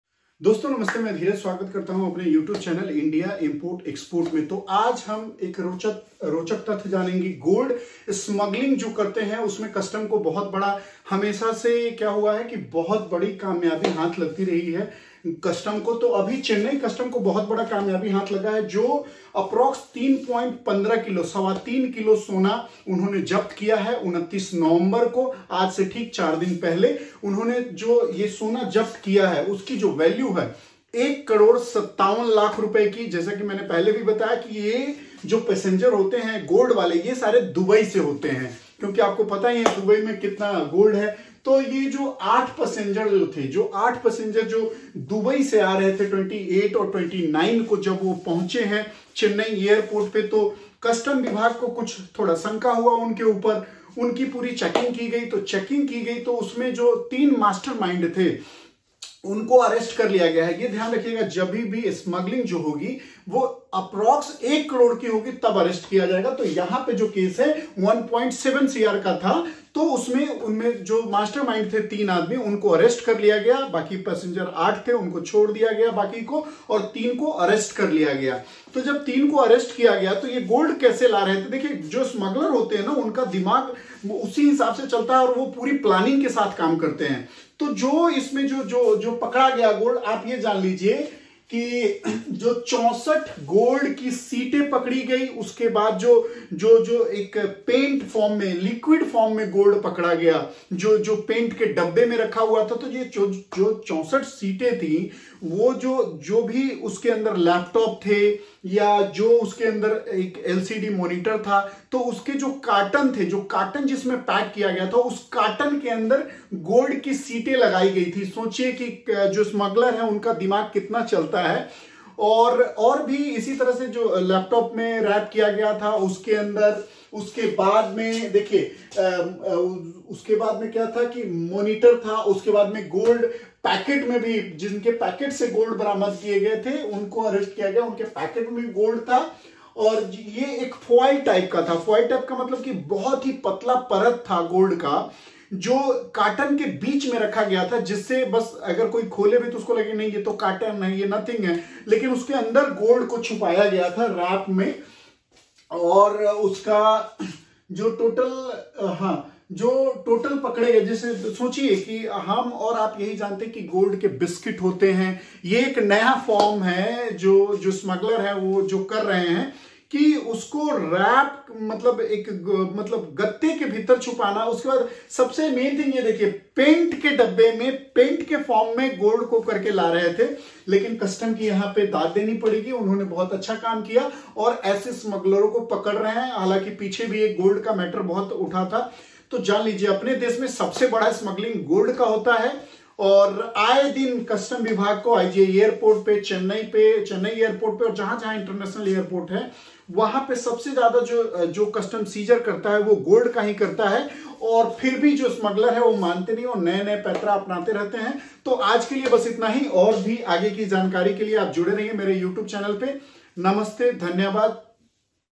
Gold smuggler Vs India Customs || International Airport pe Indian Customs kafi Gold size krti hai
Gold smugglers vs indian customs